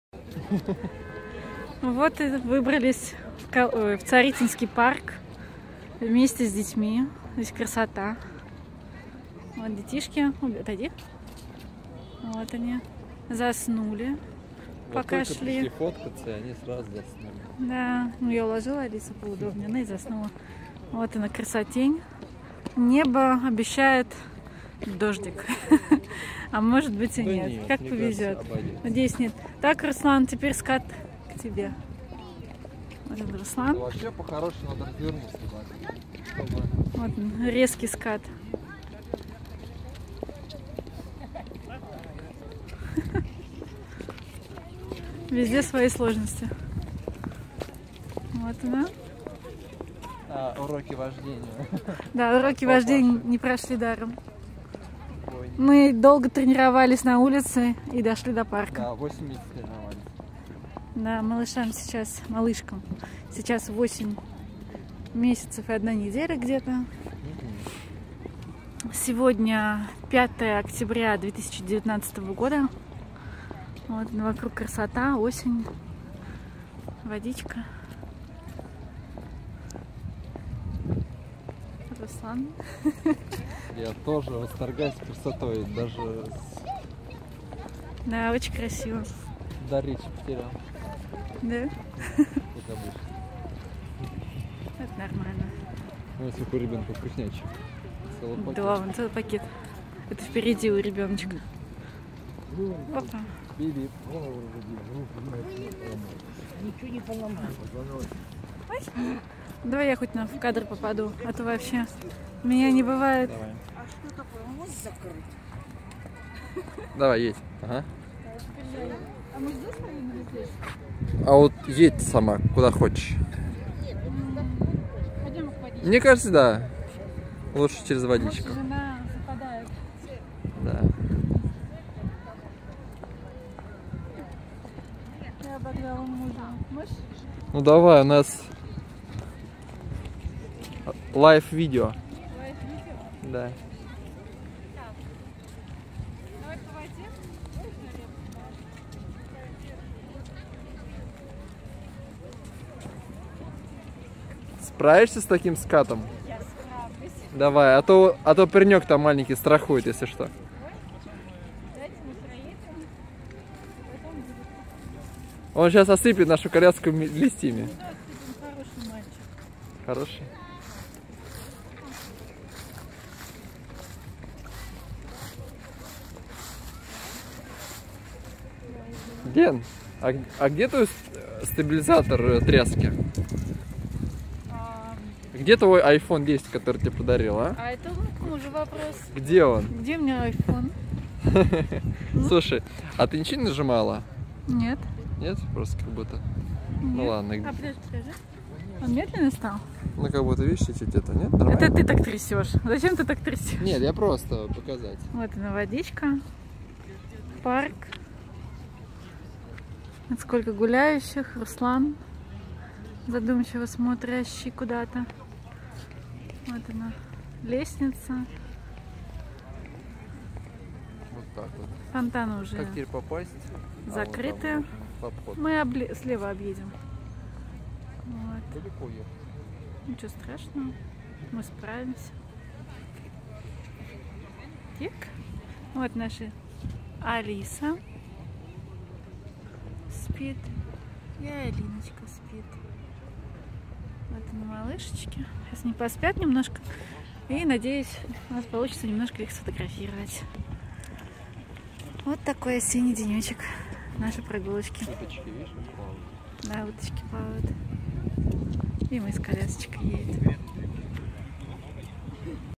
Прогулка с близняшками в парке Царицыно Elene и Ruslan
Осень в парке Царицыно.
Решили провести выходной с детьми в парке в этот красивый осенний день. :)
Наши близняшки в прогулочной коляске. В этот  день было тепло и гулять с детьми было одно удовольствие. Жаль мало получилось. Хотелось бы побольше ) Но муж сказал что детки замерзли и ДОМОЙ) ощущение как будто как котенка из парка утаскивают. из такой красоты сложно уходить. Хочется гулять там еще и еще и еще!!!